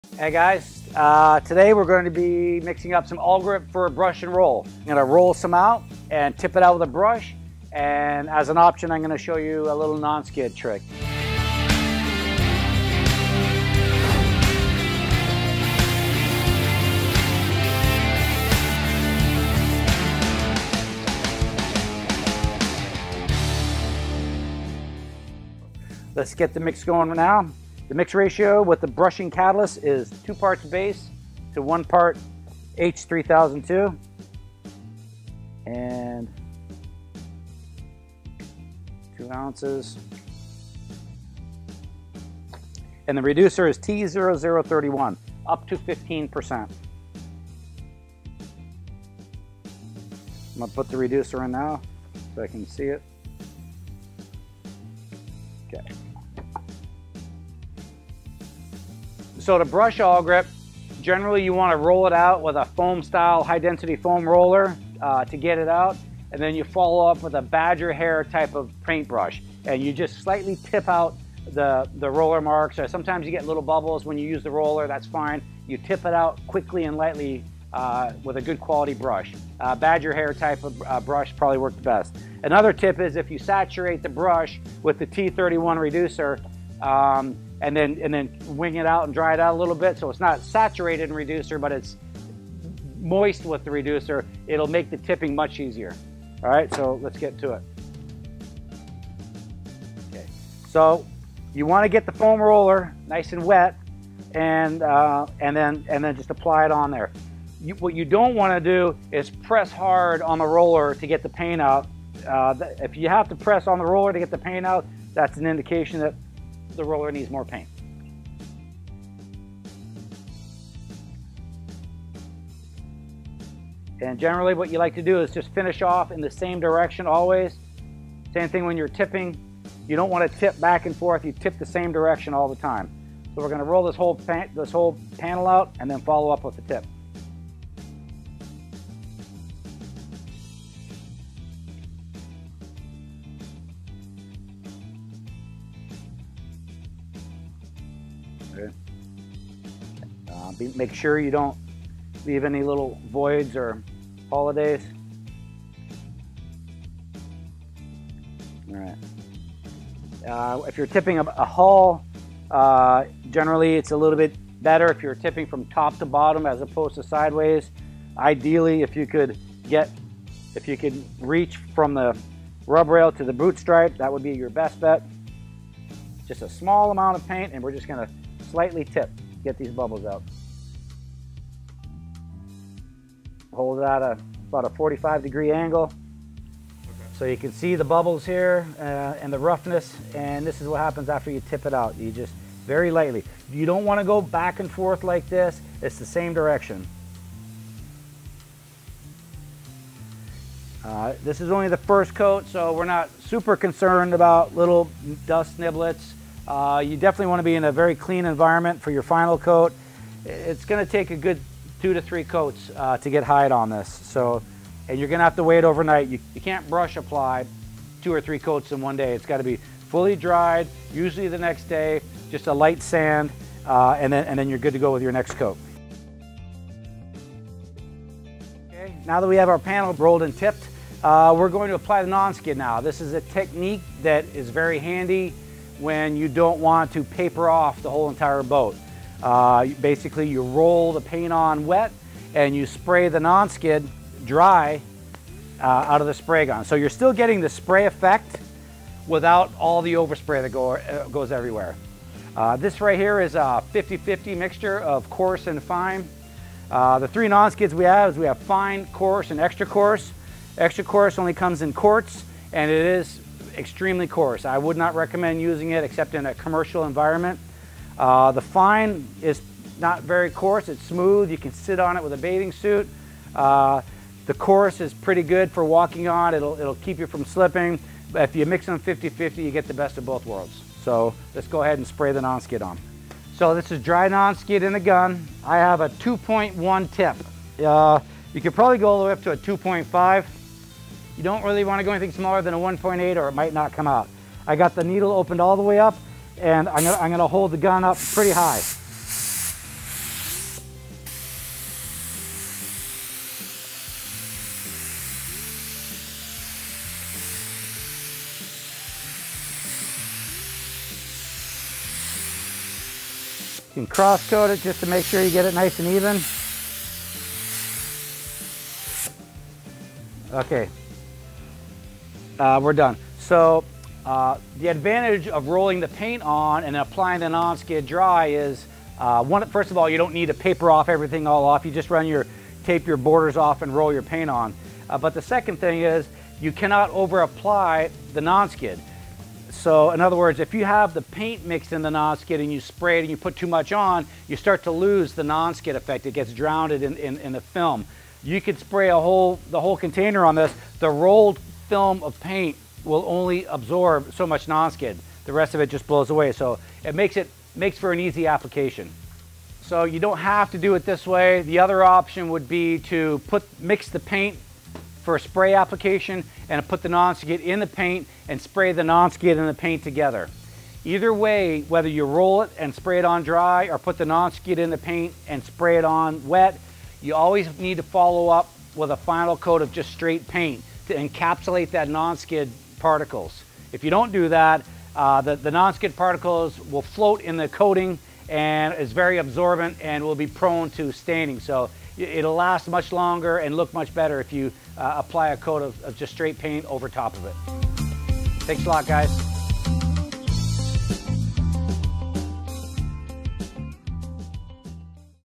Roll & Tip Awlgrip : Applying Non Skid over wet paint
In this video Bob Lalonde Shows us how to roll and tip Awlgrip. He then demonstrated how to shoot dry non skid over the wet paint. This gives you a great non skid finish.